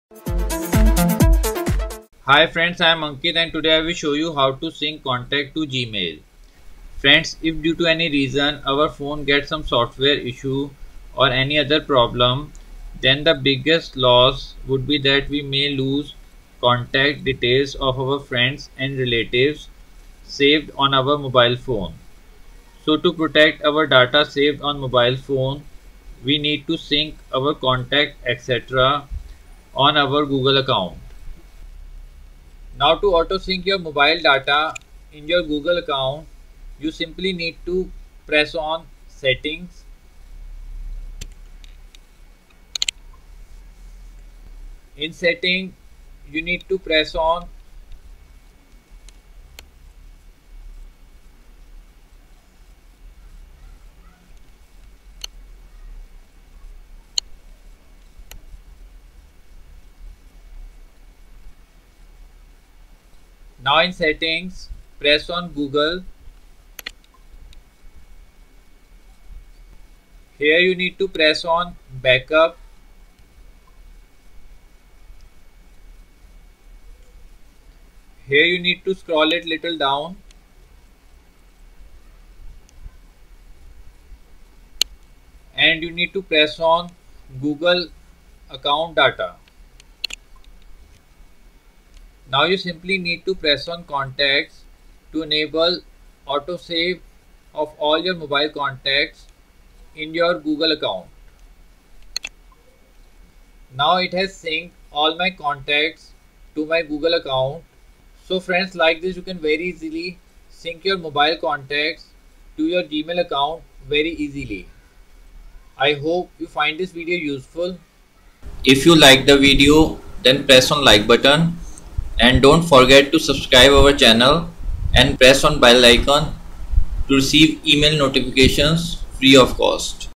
How to Import Phone Contacts to Gmail
In this video we have shown How to Import Phone Contacts to Gmail Step by Step